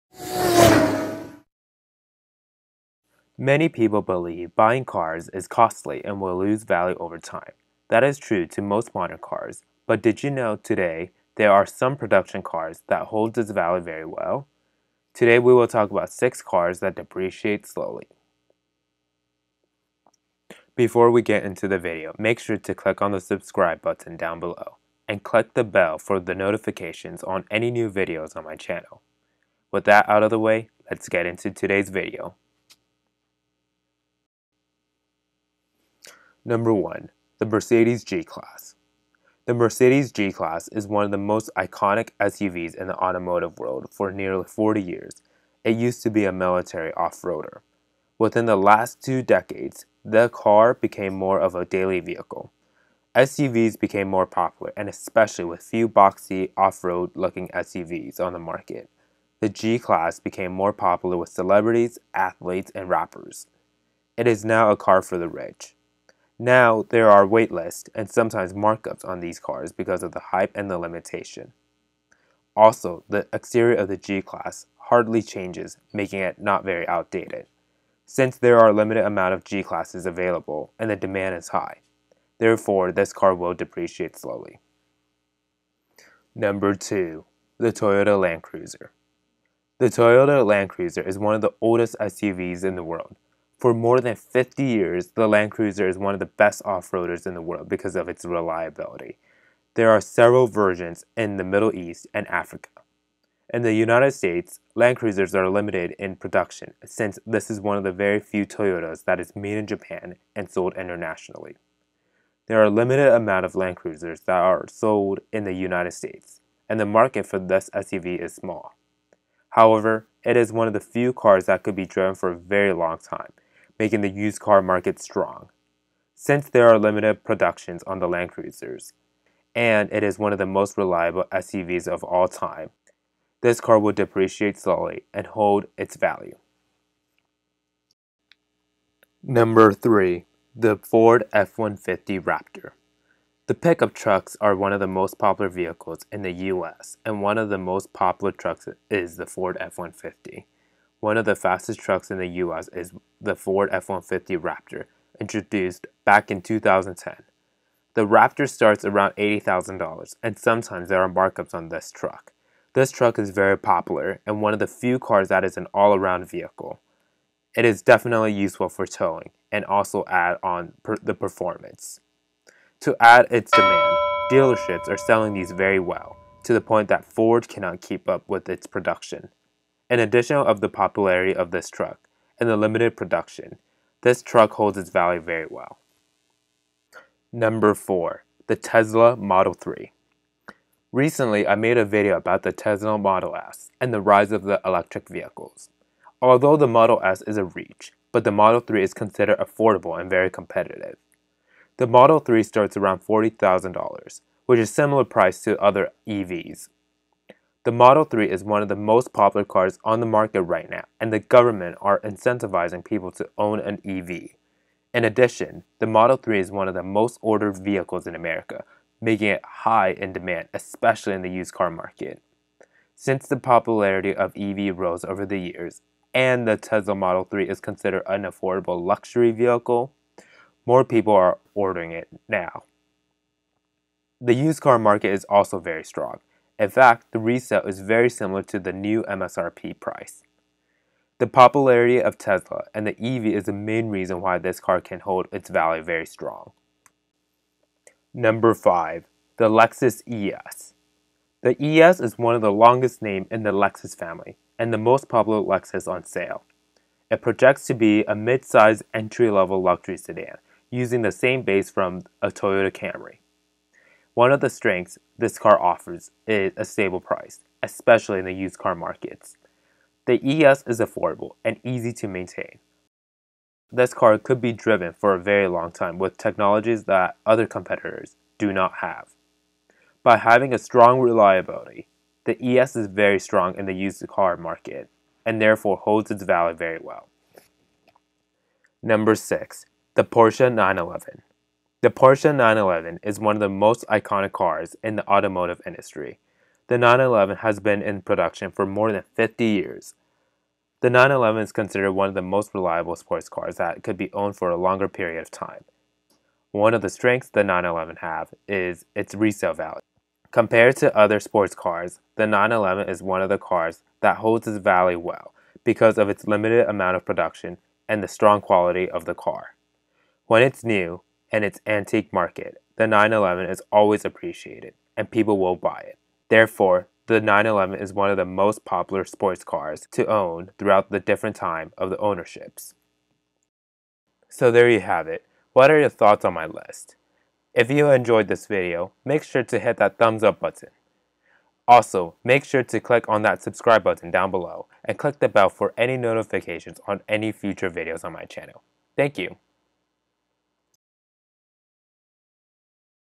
6 Cars That Holds its Value Well
Hey Everyone.  Most cars will depreciate.  However, there are some production cars that holds very well.  Here is a video that talks about the 6 cars that depreciate slowly and hold its value well.

Powered by : Pixabay & Pexels stock video

The images and videos are used for illustrative and exemplification reasons, also quoting in order to recombine elements to make a new work.